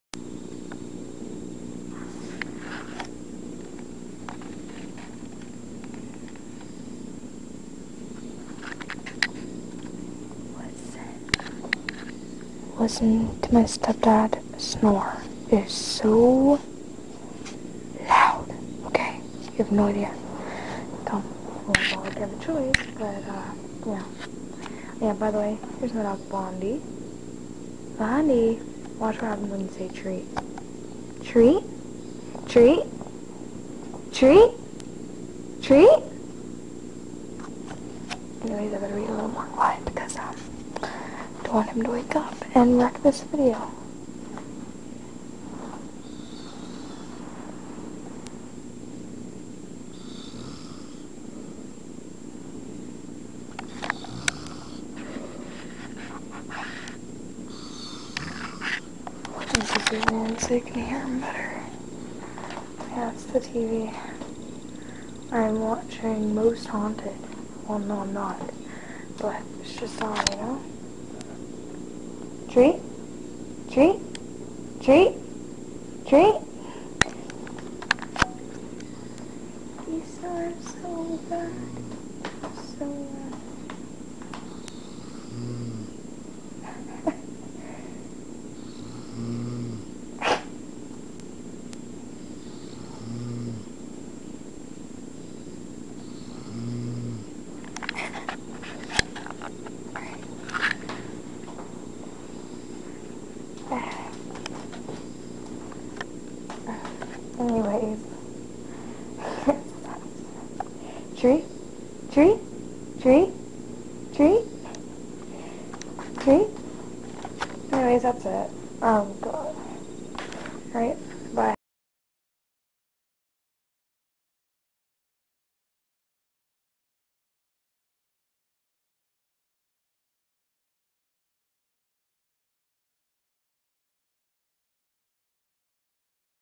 Gary Snoring- Part 1
My step-dad snoring louder then a friggin motorcycle!...well not really but u know what I mean!